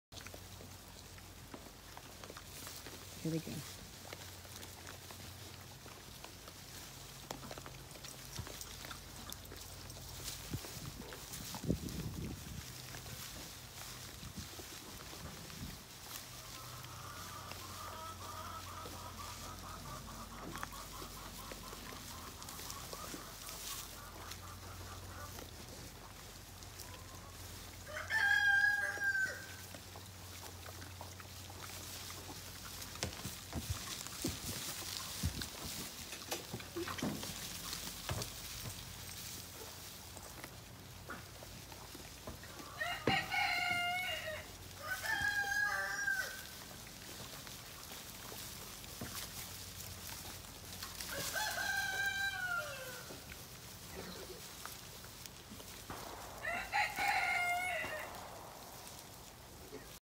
Boss Bacon
Will papa pig maintain discipline among the boys?